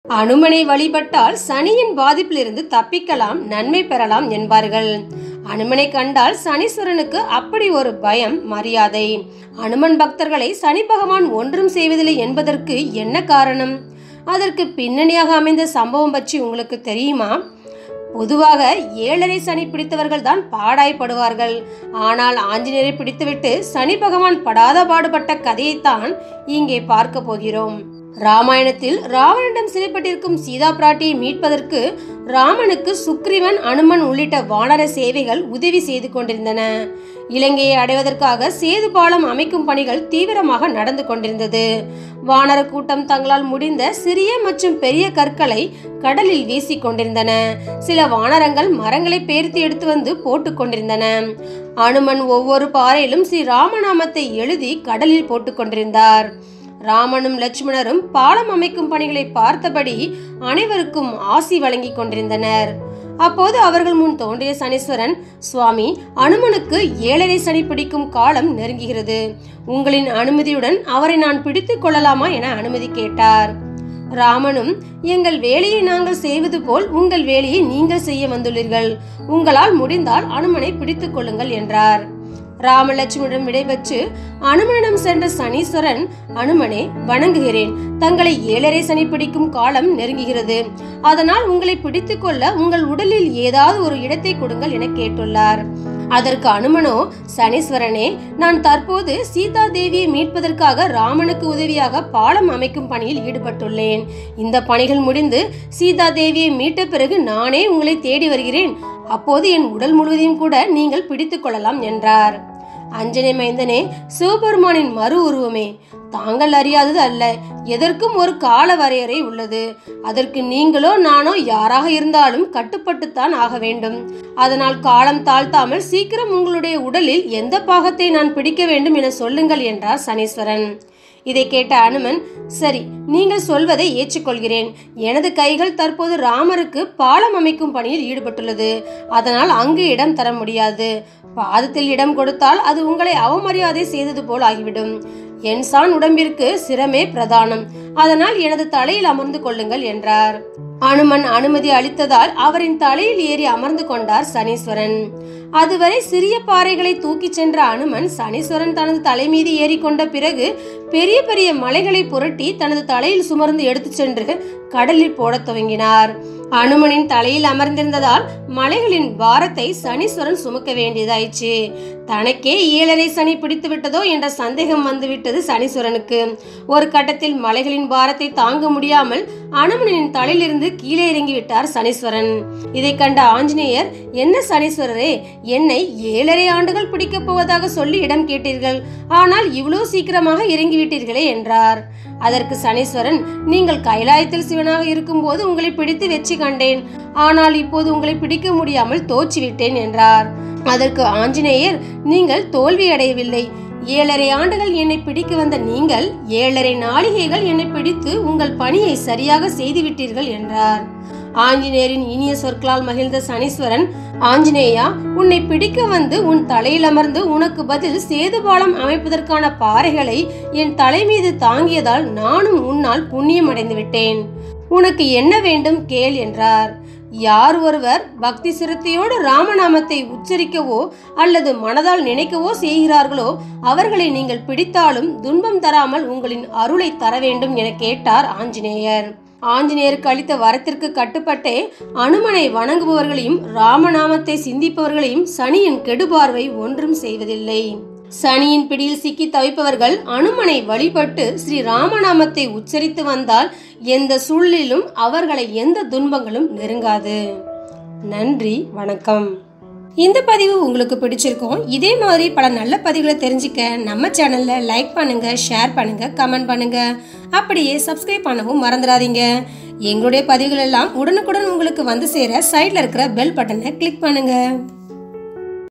ஆஞ்சநேயரிடம் அகப்பட்டுக் கொண்ட சனீஸ்வரன் | அனுமனுக்கு ஏழரை சனி பிடிக்கும் காலம் நெருங்குகிறது
சனியின் பிடியில் சிக்கி தவிப்பவர்கள், அனுமனை வழிபட்டு ஸ்ரீ ராம நாமத்தை உச்சரித்து வந்தால் எந்த சூழலிலும் அவர்களை எந்த துன்பங்களும் நெருங்காது..